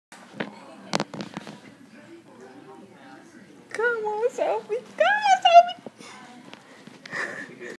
Sophie cute puppy high stepping in her shoes
Beagal puppy first time walking in shoes